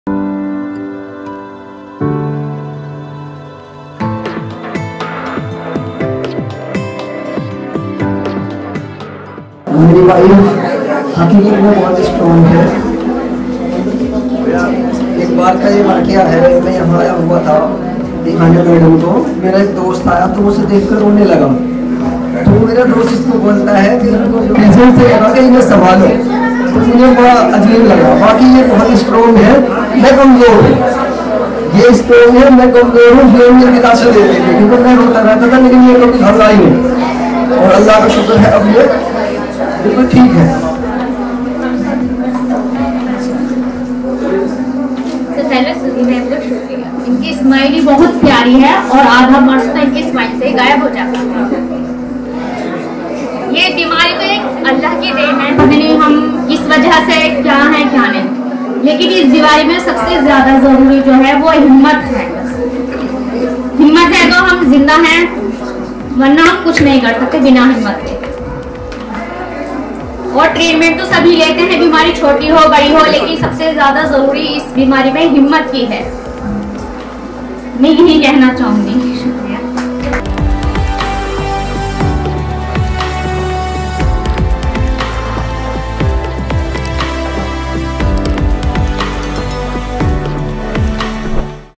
ब्रैस्ट कैंसर से जीत: सरवरी जी की प्रेरक यात्रा।
Patient Feedback.
Thank you very much for coming to our camp.